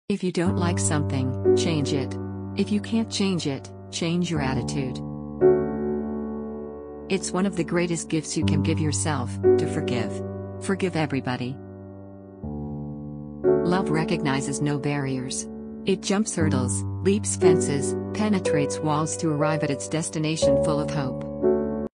OF MAYA ANGELOU'S BEST QUOTES TO INSPIRE
Maya Angelou was an  writer, actress, screenwriter, dancer, poet, and civil rights activist best known for her 1969 memoir, I Know Why the Caged Bird Sings, which went down in literary history as the first non-fiction bestseller by an African. an american a woman Angelou has received numerous awards throughout her career, including two NAACP Image Awards for Outstanding Writing (Nonfiction) in 2005 and 2009. @Of Quote chanel